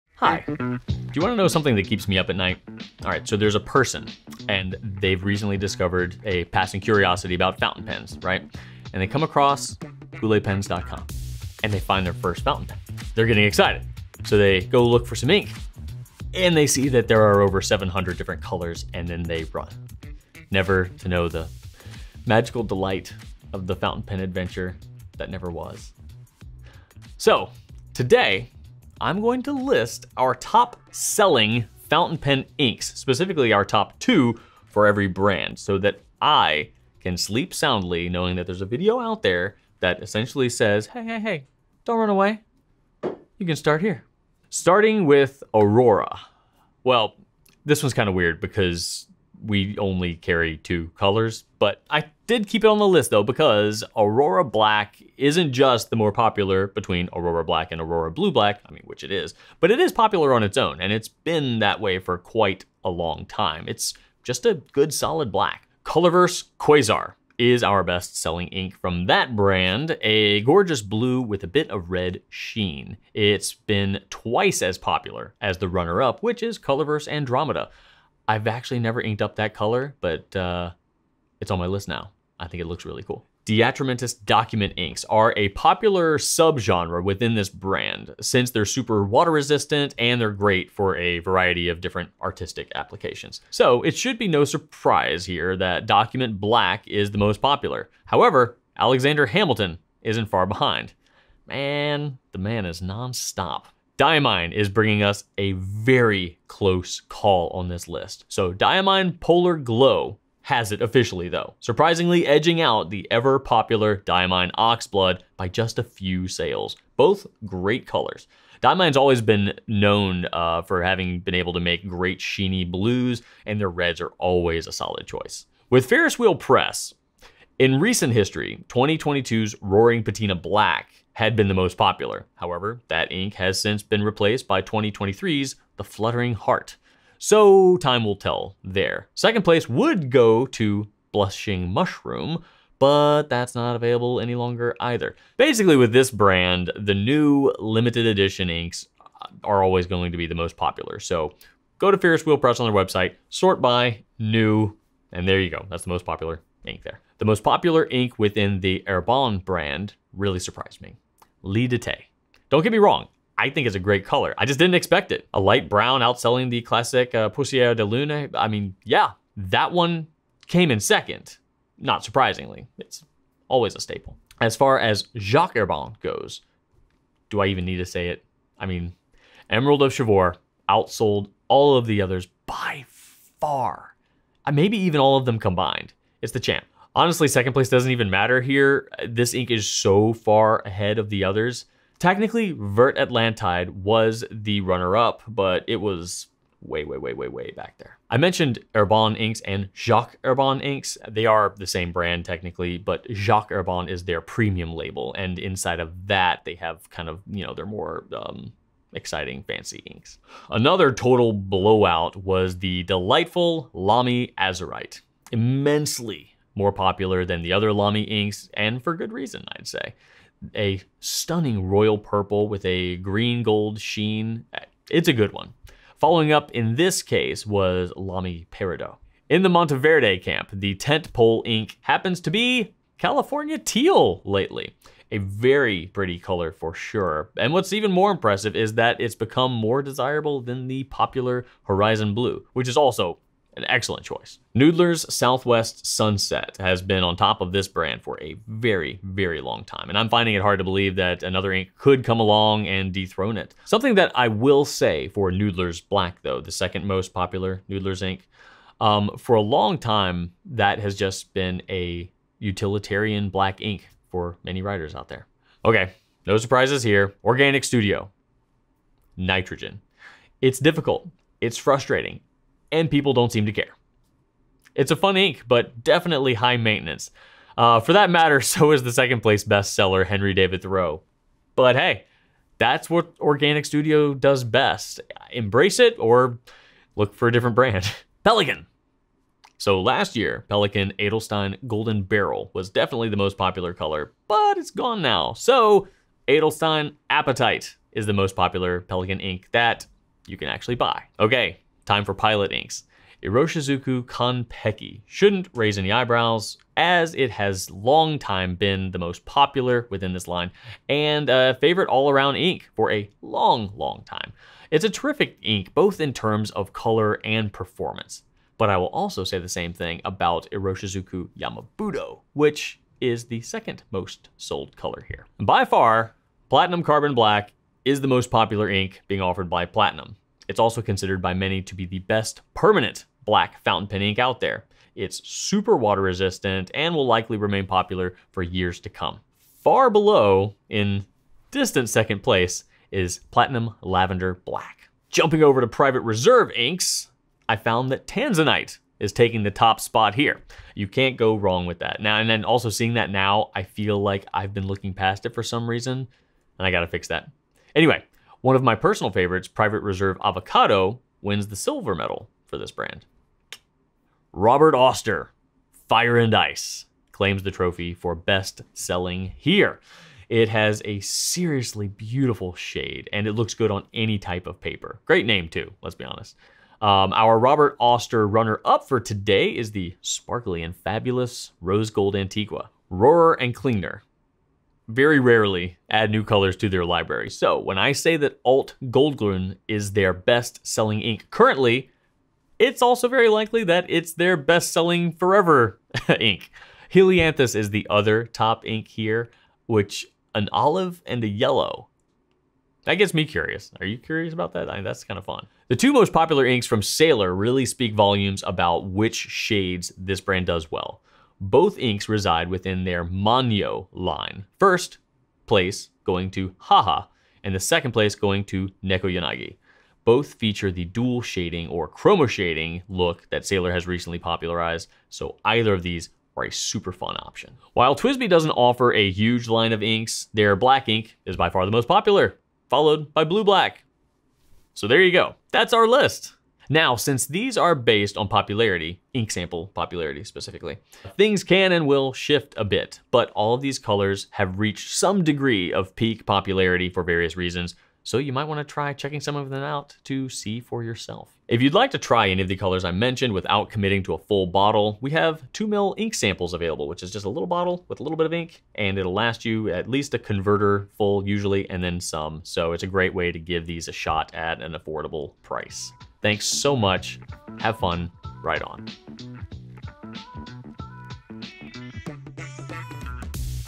The Best Selling Inks of 19 Different Brands
If the massive amount of fountain pen ink options are overwhelming you, start here! Drew reviews our current most popular inks across 19 different brands!

TIMESTAMPS:
0:00 Intro
0:44 Aurora
1:05 Colorverse
1:23 De Atramentis
1:43 Diamine
2:05 Ferris Wheel Press
2:36 Herbin
2:54 Jacques Herbin
3:30 LAMY
3:48 Monteverde
4:04 Noodler's
4:25 Organics Studio
4:47 Pelikan
5:00 Pilot
5:24 Platinum
5:44 Private Reserve
6:03 Robert Oster
6:22 Rohrer & Klinger
6:45 Sailor
7:07 TWSBI